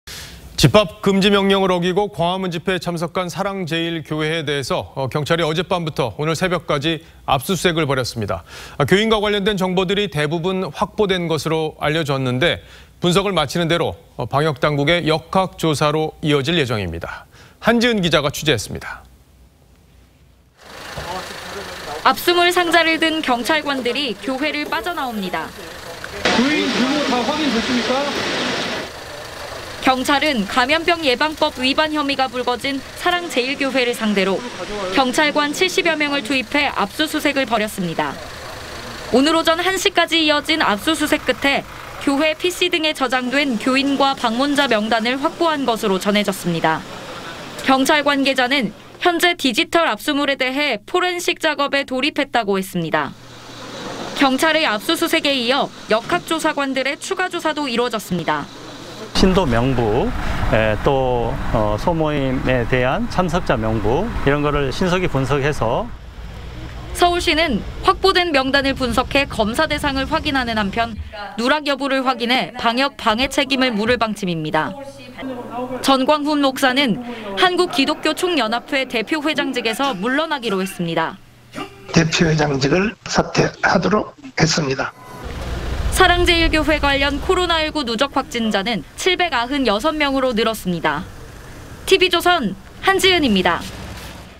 경찰, 사랑제일교회 4시간 압수수색…교인·방문자 명단 확보
집합금지 명령을 어기고 광화문집회에 참석한 사랑제일교회에 대해 경찰이 어젯밤부터 오늘 새벽까지 압수수색을 벌였습니다. 교인과 관련된 정보들이 대부분 확보된 것으로 알려졌는데, 분석을 마치는 대로 방역당국의 역학조사로 이어질 예정입니다.